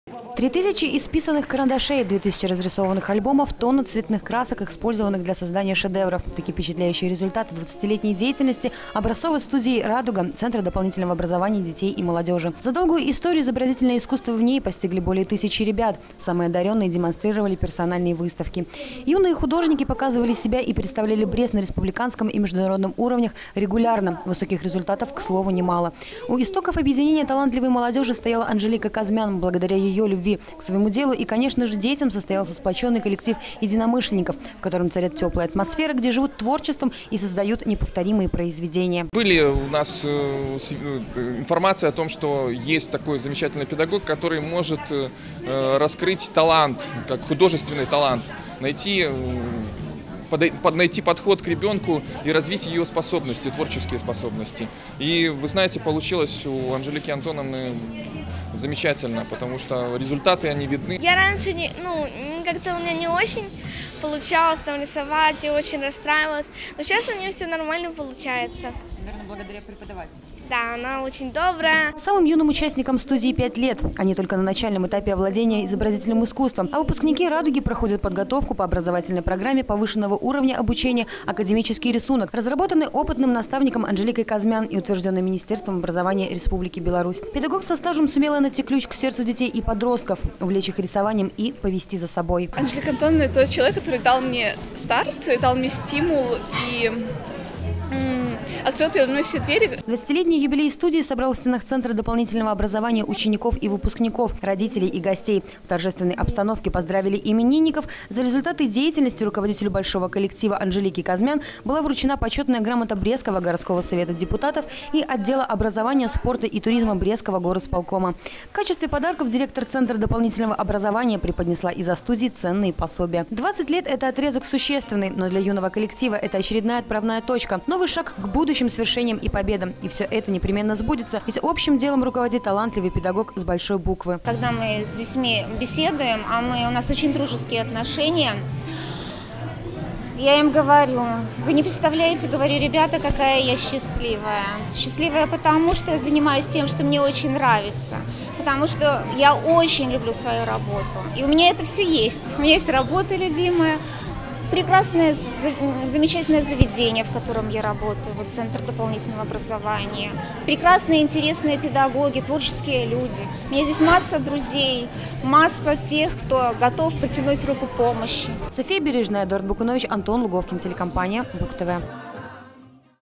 2017-02-07 г. Брест. 25-летие студии «Радуга». Новости на Буг-ТВ.
В центре дополнительного образования детей и молодёжи города Бреста состоялось торжественное мероприятие, посвящённое 20-тилетнему юбилею образцовой студии изобразительного и декоративно-прикладного искусства «Радуга». За время её существования из-под крыла профессионала своего дела, педагога Анжелики Козмян было выпущено немало талантов. Многие из них пошли по стопам своего руководителя и связали жизнь с искусством. Радостная встреча в кругу воспитанников и их родителей по случаю круглой даты стала поводом подвести итоги деятельности, вспомнить знаковые моменты.